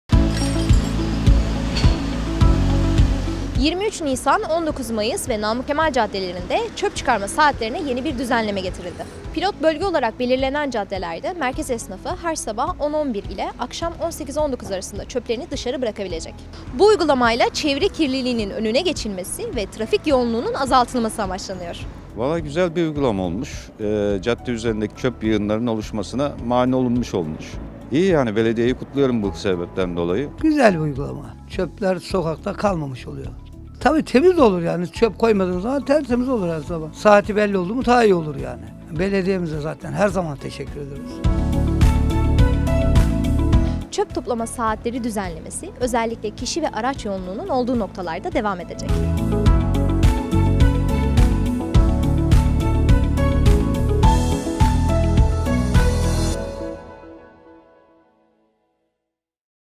Pendik Çarşı'da Çöp Çıkarma Saatlerine Düzenleme Yapıldı
Pendik Çarşı'daki bazı caddelerimizde çöp çıkarma saatlerine yeni düzenleme getirdik. Yeni uygulamamızı tüm çarşı bölgemizde yaygınlaştırmayı hedefliyoruz.